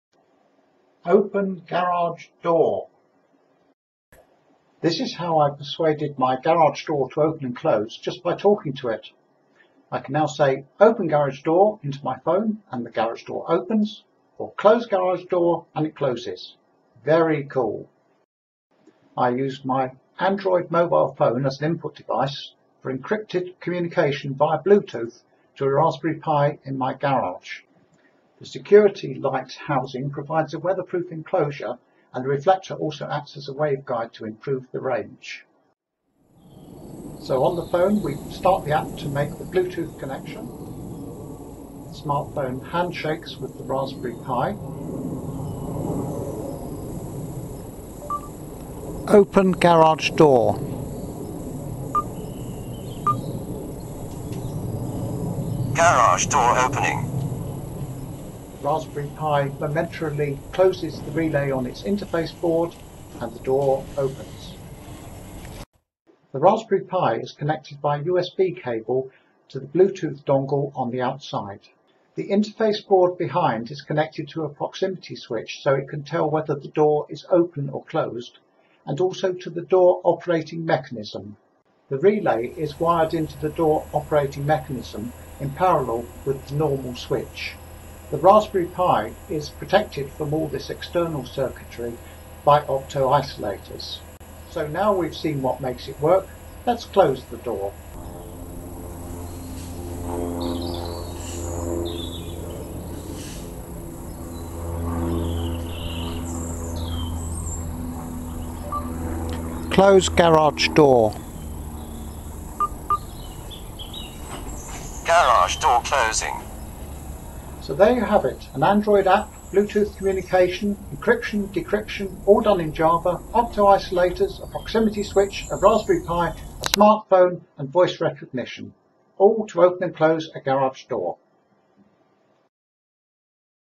Open garage door !
How to open a garage door with an android phone, a little Java, bluetooth and a Raspberry Pi.